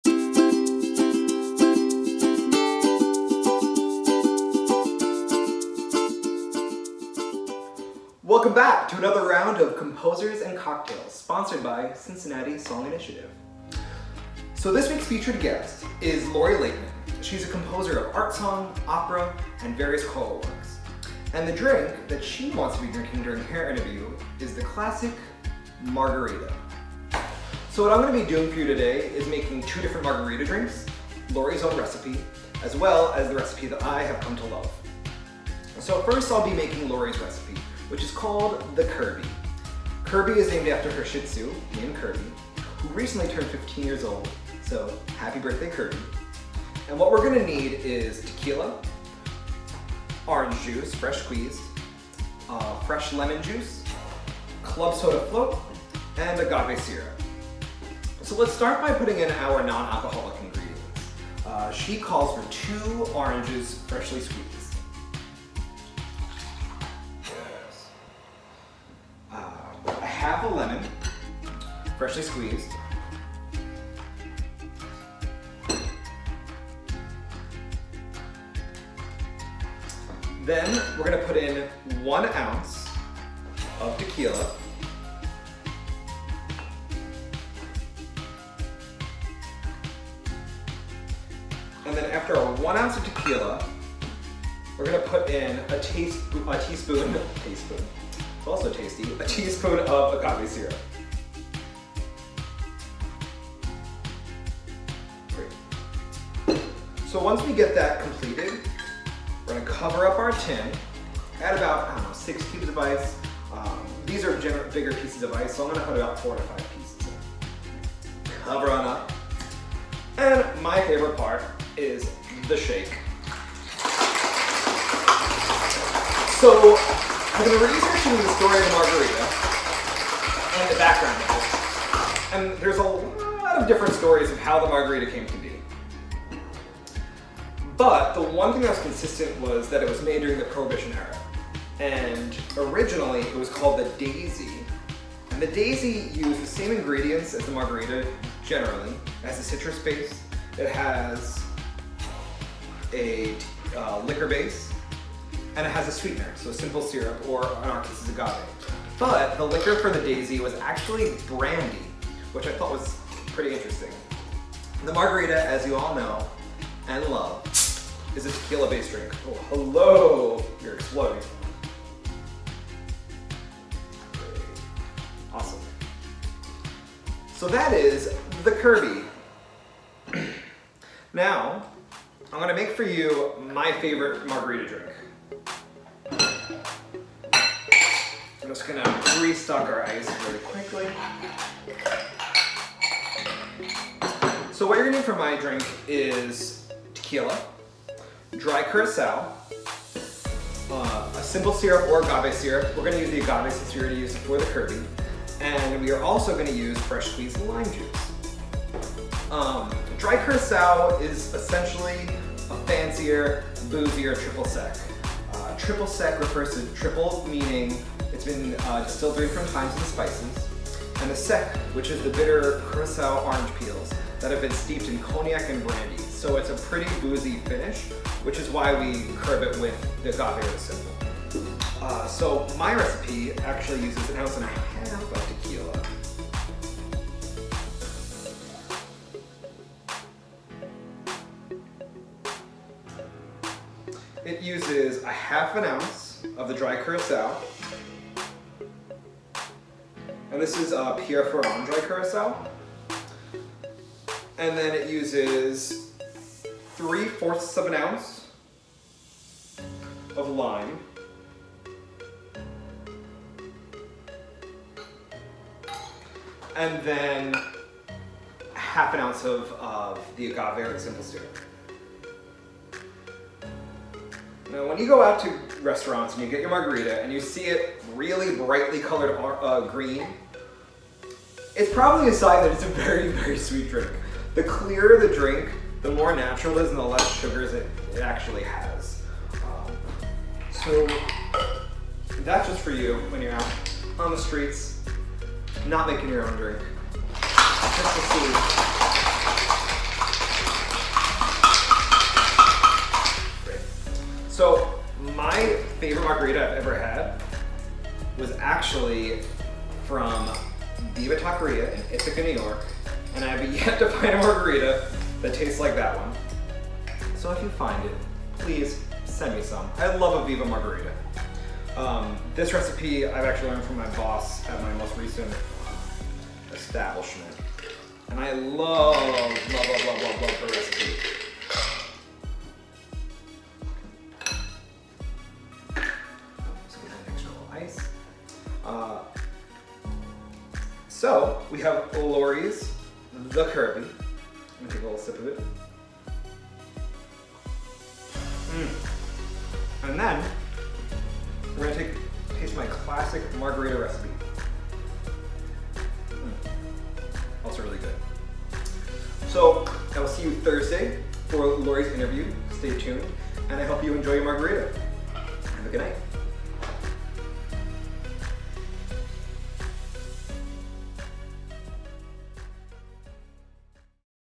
Lori Laitman's Margarita, "The Kirby"
Bartender Matt helps us prepare Lori Laitman's favorite cocktail, The Kirby margarita, before tuning in to her happy hour chat.